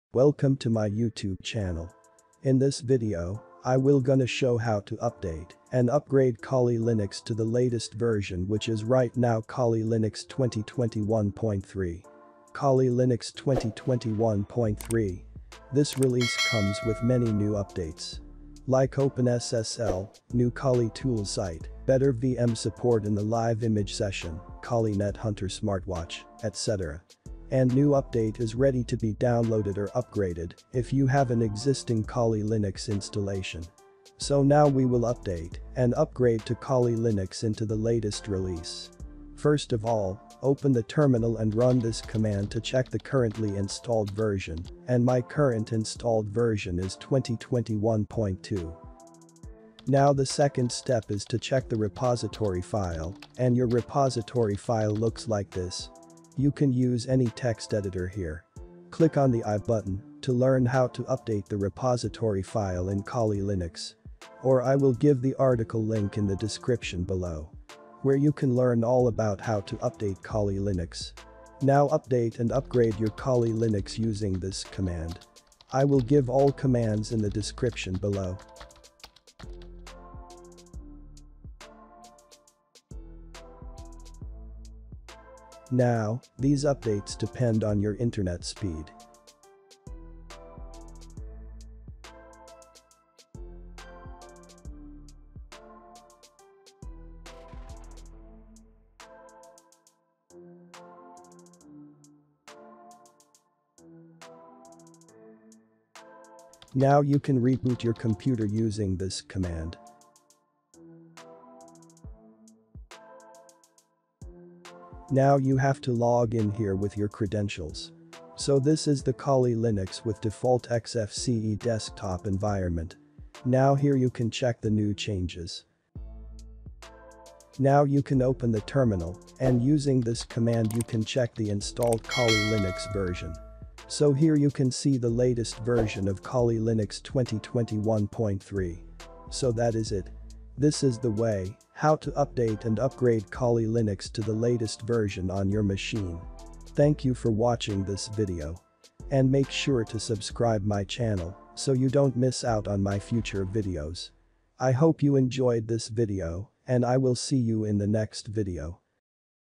How to Update Kali Linux into the Latest Release | Kali Linux 2021.3
In this video, I will show How to Update Kali Linux into the Latest Release Kali Linux 2021.3 step by step. So finally we have a new release Kali Linux 2021.3 is out. So how to update and upgrade from the older version to this one. This release comes with many new updates and tools. Like OpenSSL, New Kali Tools site, Better VM support in the live image session, KaliNethunter smartwatch, etc.

☑ Commands:
➥ $ sudo apt update
➥ $ sudo apt full-upgrade -y

That is it! This is the Way How to Update Kali Linux 2021.2 into Kali Linux 2021.3